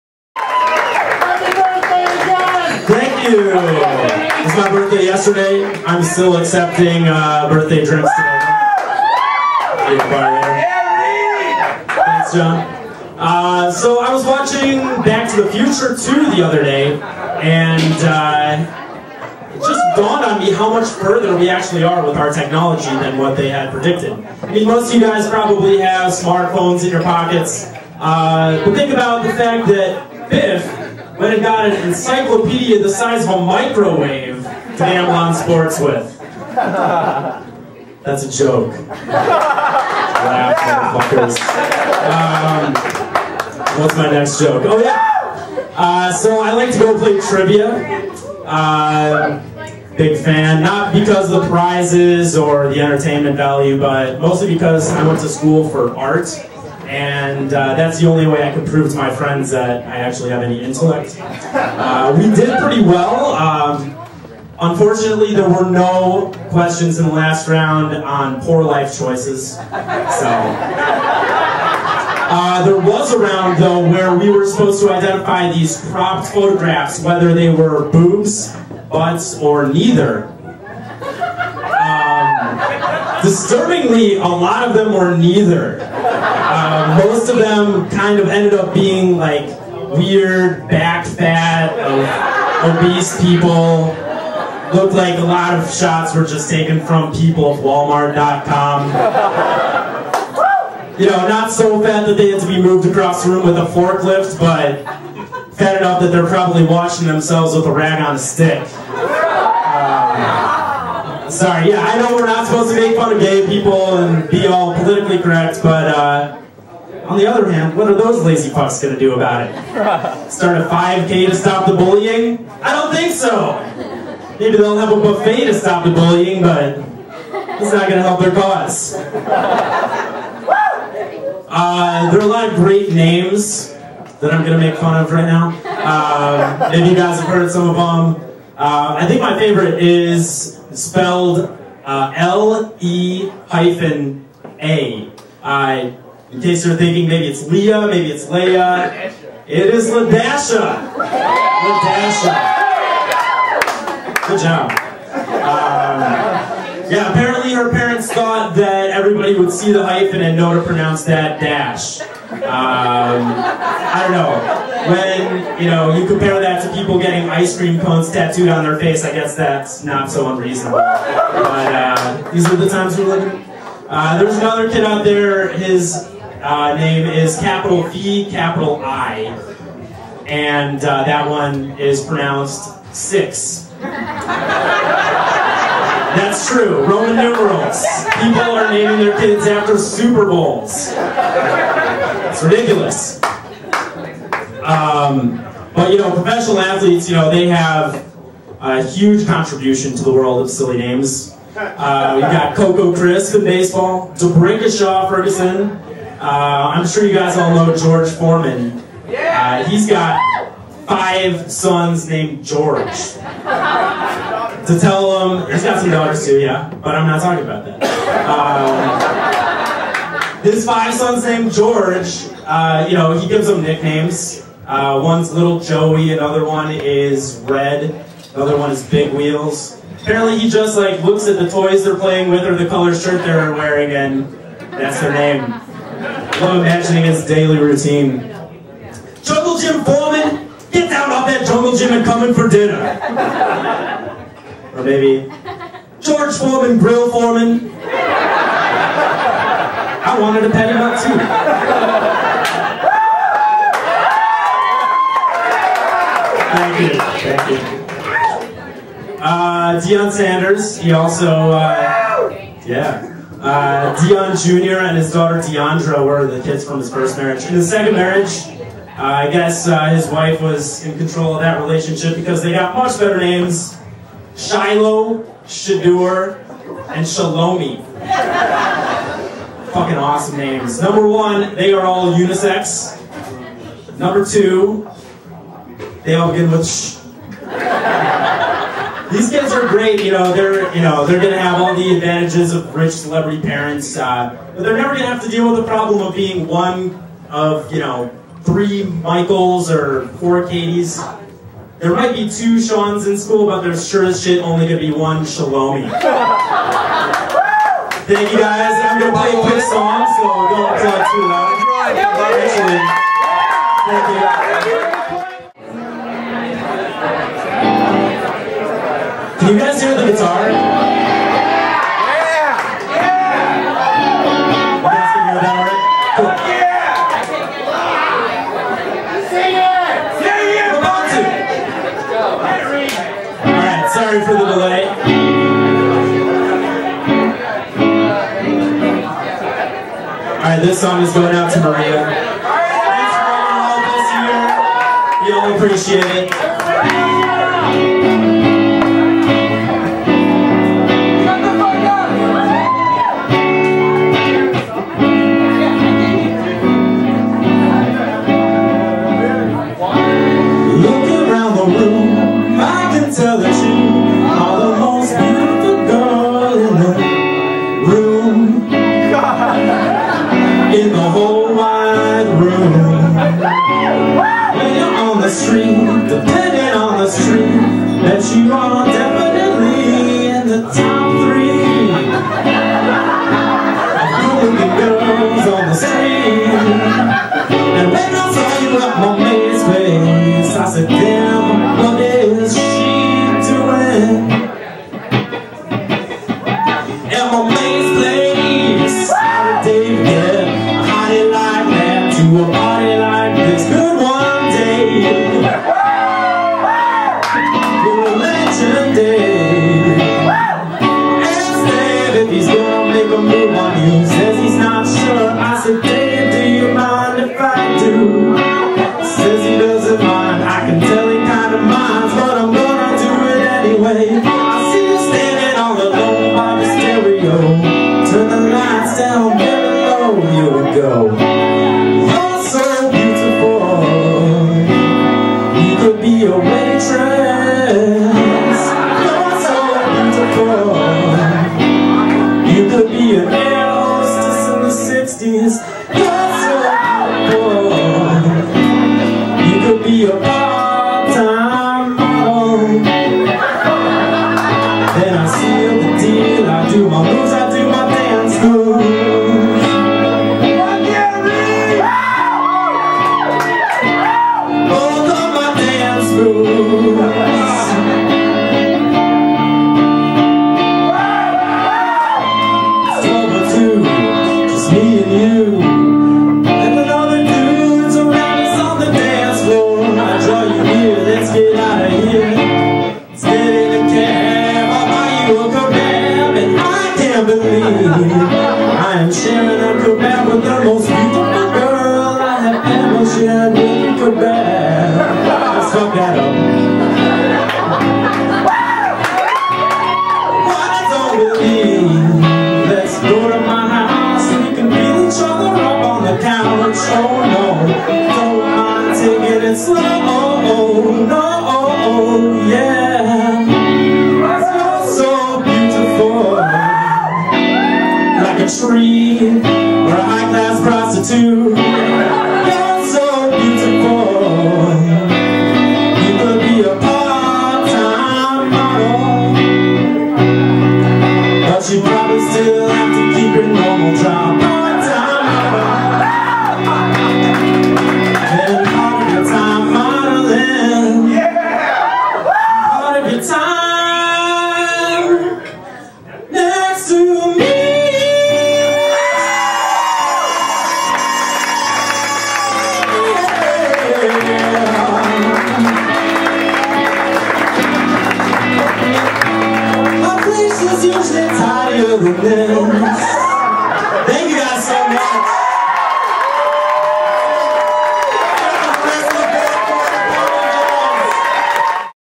Reed Conway - Comedy Night
At Chicago Portfolio School, students give their final exams standing up.